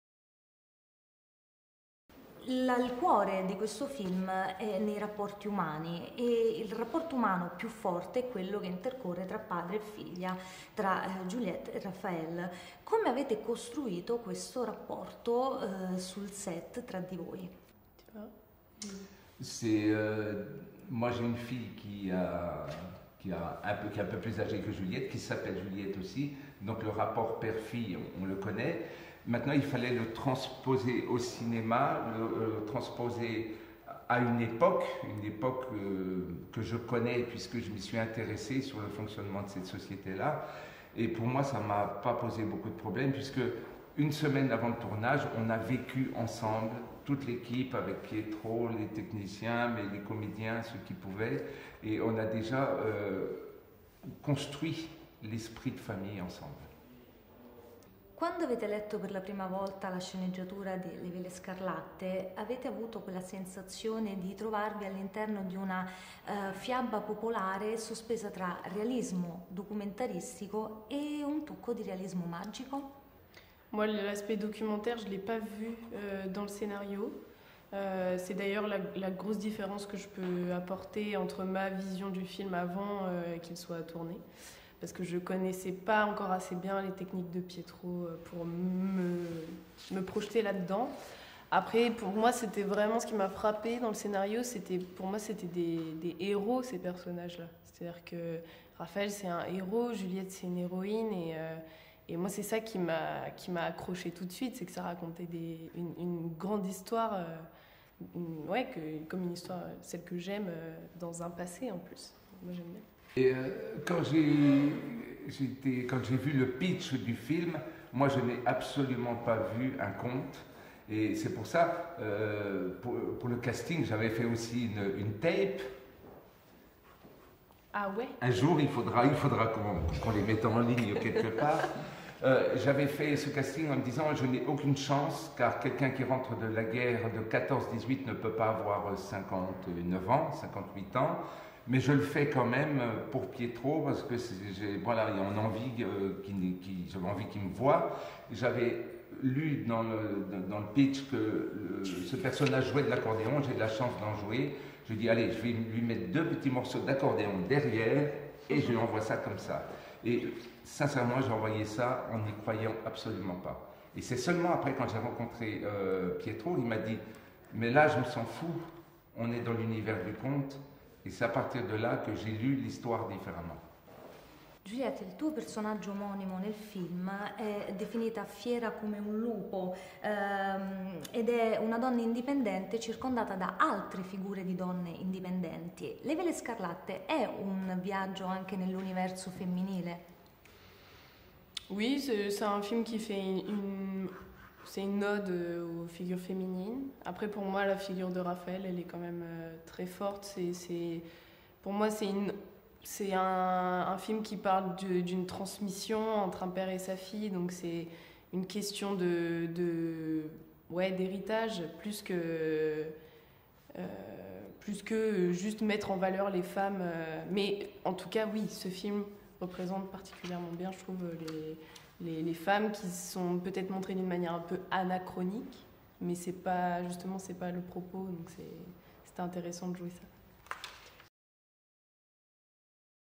Le vele scarlatte - Intervista a Juliette Jouan e Raphaël Thiéry
Moviestruckers.it intervista gli attori Juliette Jouan e Raphaël Thiéry in occasione della presentazione alla stampa de Le vele scarlatte di Pietro Marcello, dal 12 gennaio al cinema grazie a 01Distribution

Intervista a cura di Ludovica Ottaviani